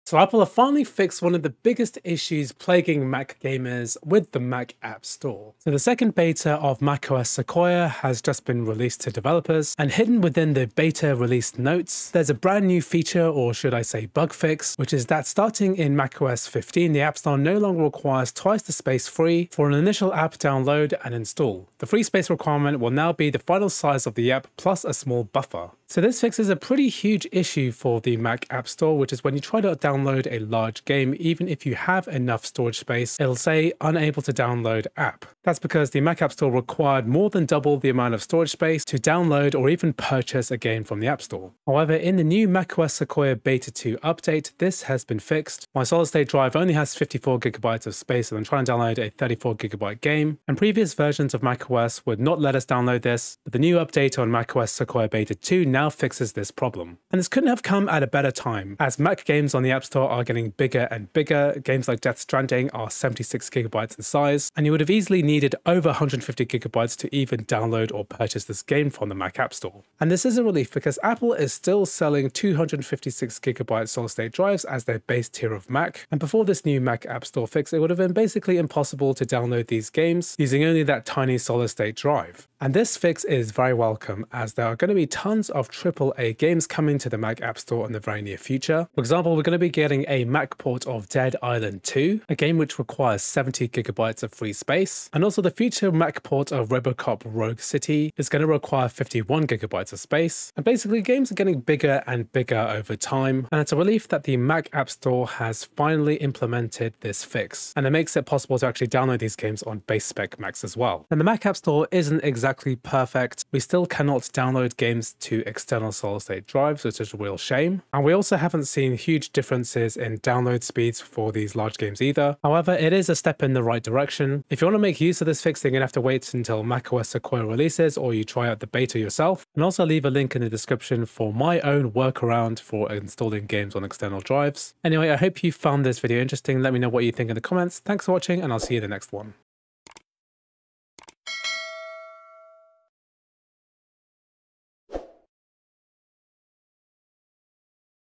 Apple fixes Mac App Store double storage issue for gamers!
Quick video about how Apple finally fixed the double storage issue with the Mac App Store.